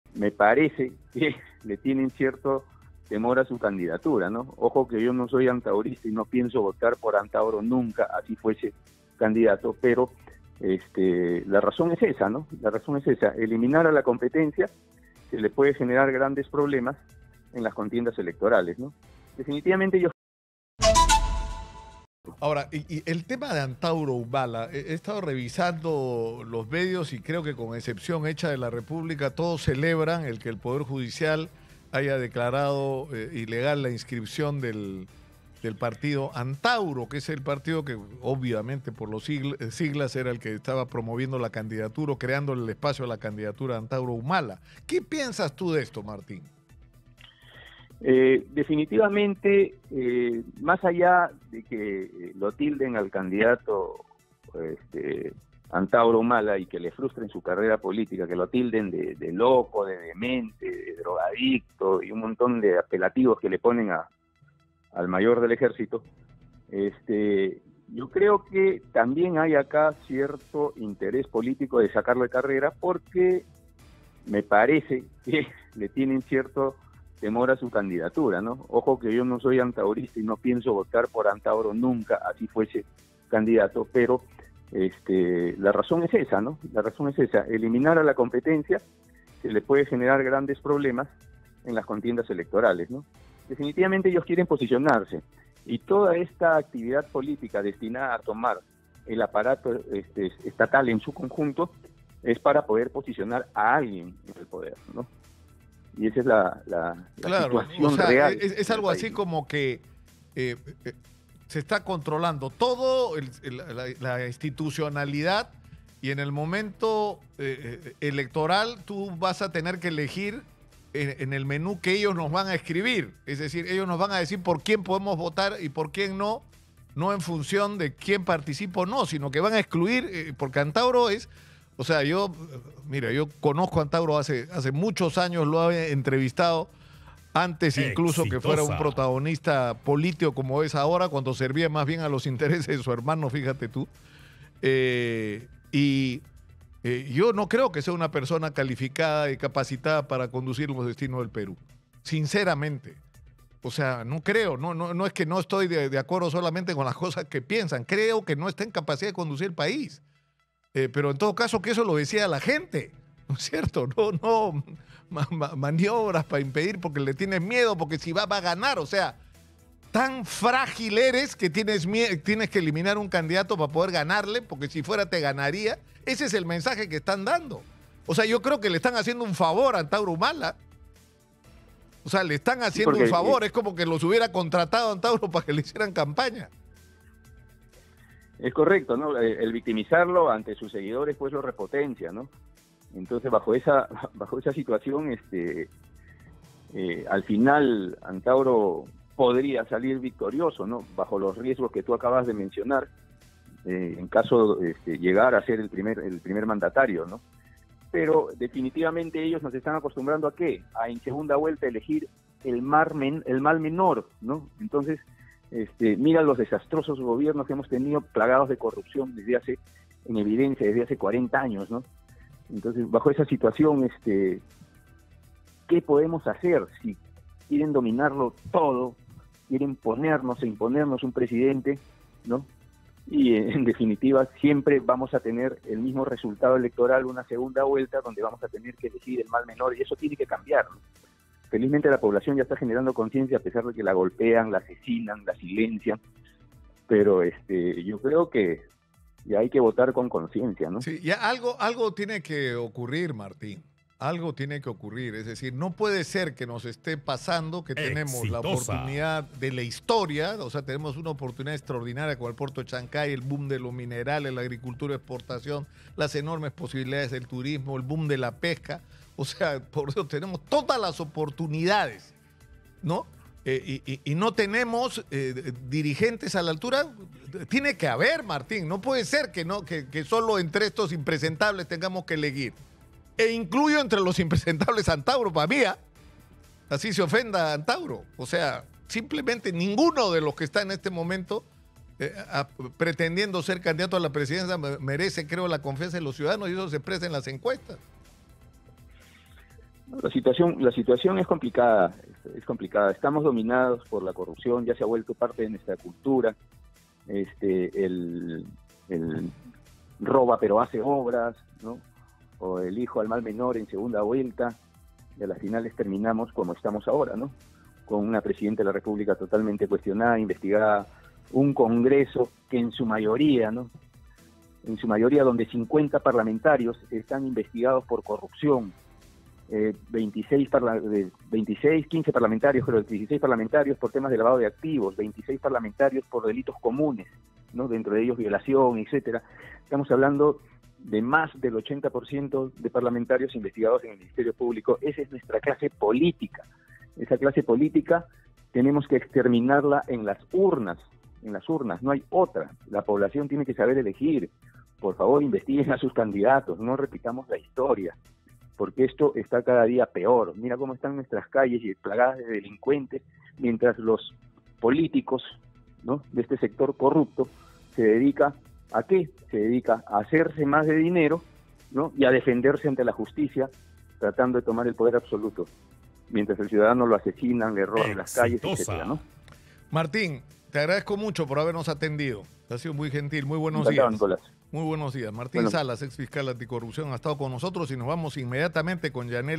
🔴🔵Buscan sacar a Antauro Humala de carrera, asegura exfiscal anticorrupción
En diálogo con Exitosa, el exfiscal anticorrupción Martín Salas cuestionó la resolución que ordena la disolución de A.N.T.A.U.R.O y aseguró que buscan sacar de carrera a Antauro Humala: "Le tienen temor a su candidatura".

Exitosa Perú
"Exitosa: La voz que integra al Perú"

Corporación Universal